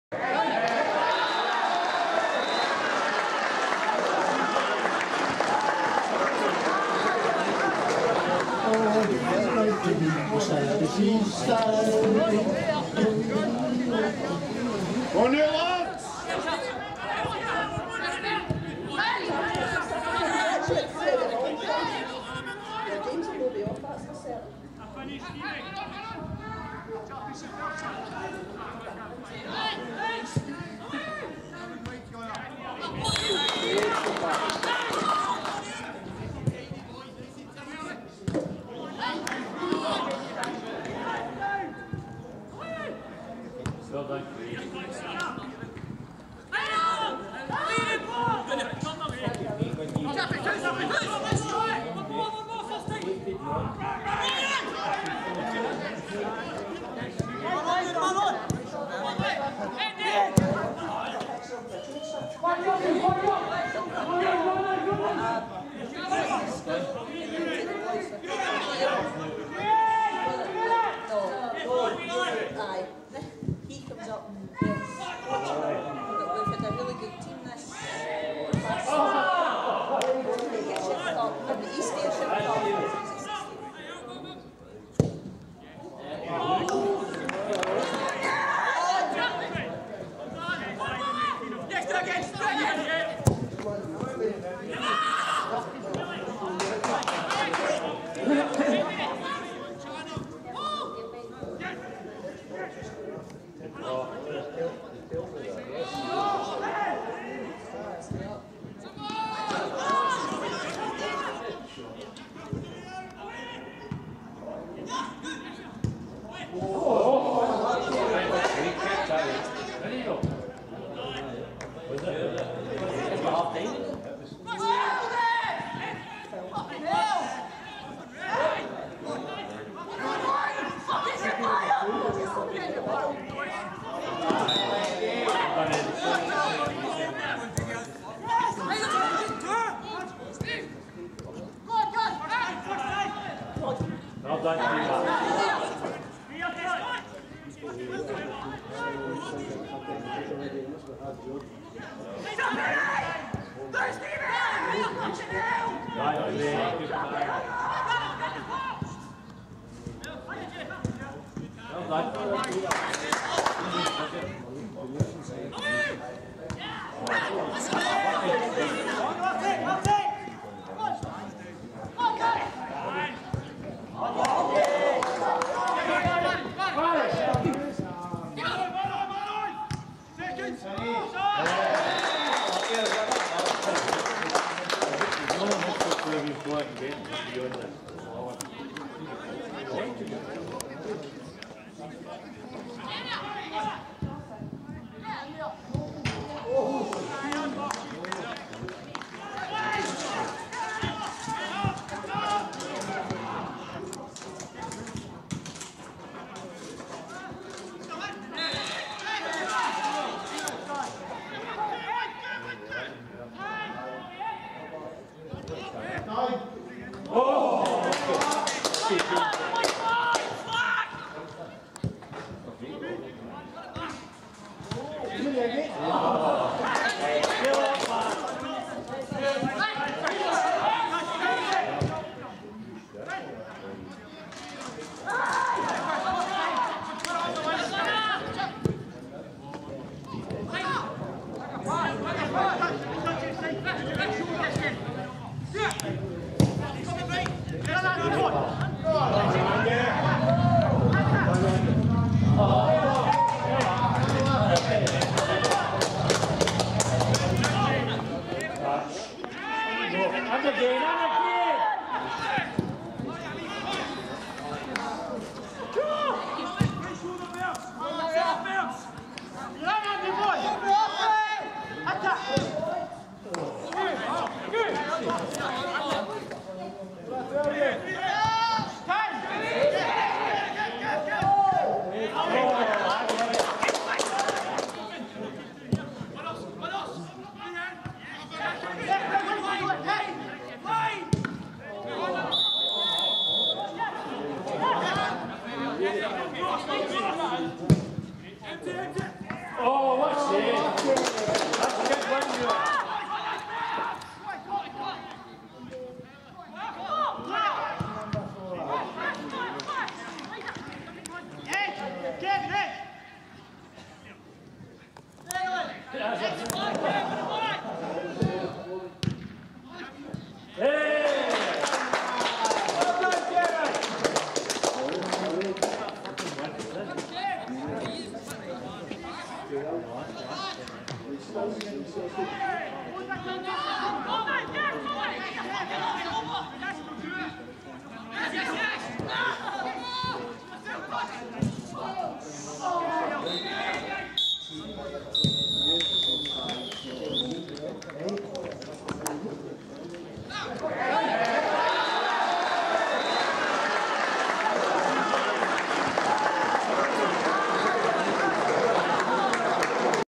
Largs Thistle 2-0 Troon, Friendly 24th July 2013
1-0 2:55
2-0 5:02

Largs welcome Troon to Barrfields in 2013-14's first friendly, with three trialists appearing for Thistle